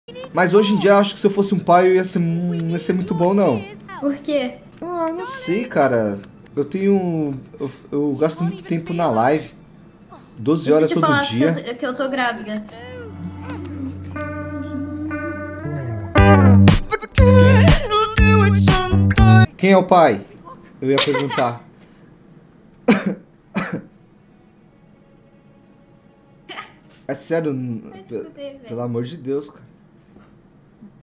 YAYAH VAZOU QUE LINDINHO VAI SER PAI?
Salve, Tribo!

A stream dele pode ser encontrada em: Twitch.tv/Deercheerup

Yayah vazou e falou que o Lindinho vai ser pai? É isso mesmo?